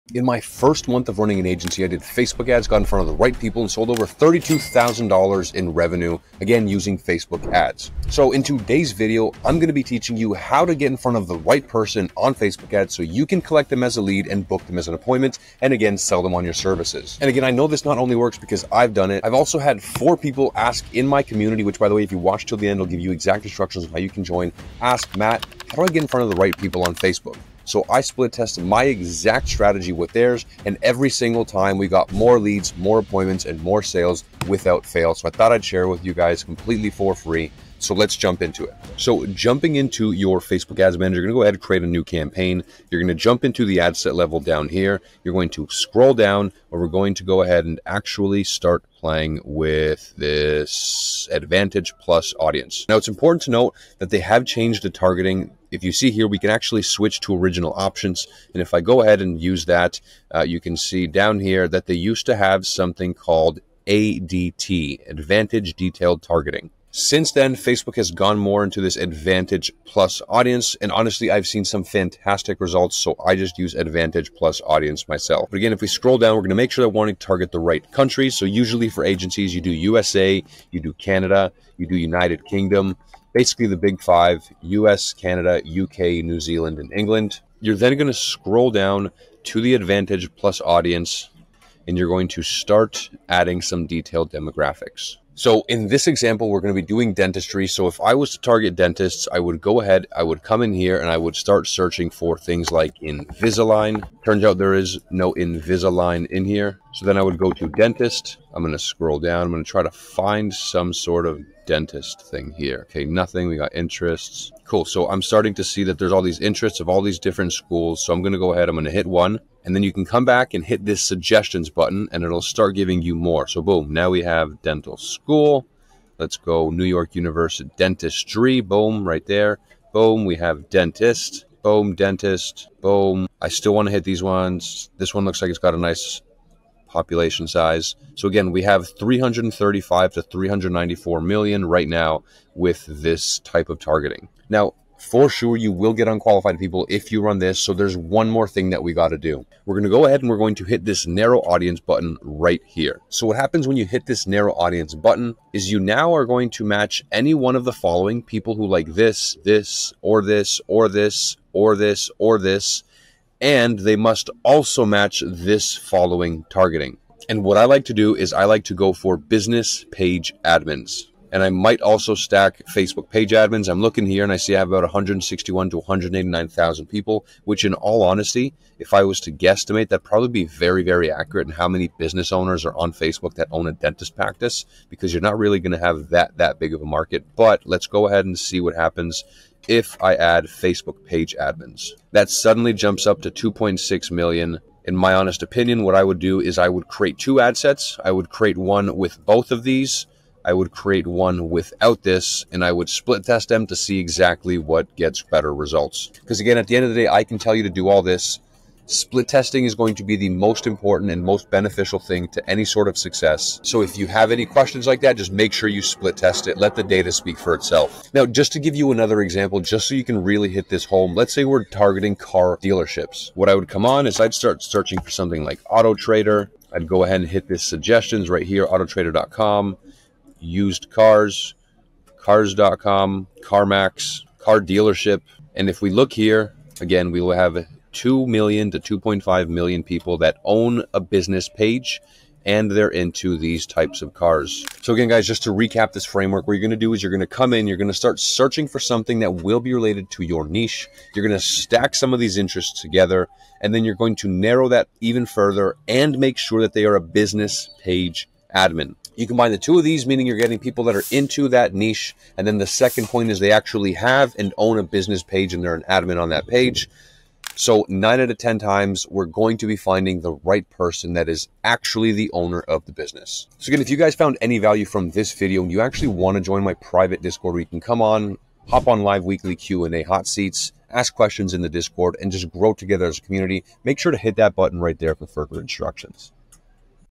My INSANE Facebook Targetting: Book Appts 2024 (SMMA)
Join My Private Skool Mastermind (Live Calls, All Templates & Resources, Ad Trainings, Automations, GHL Snapshot & More):

In this video i break down my smma's EXACT facebook ads targetting strategy moving into 2024.

This facebook ads targetting strategy allows you to get infront of business owners in your niche, so you can book more appointments in 2024 for your social media marketing agency.

Facebook ads are also one of the best ways to book more appointments for your smma in 2024 - as facebook ads work 24/7 without you.

Making sure your ads are targetting the right people at the right time is going to be super important to actually get great results for your smma.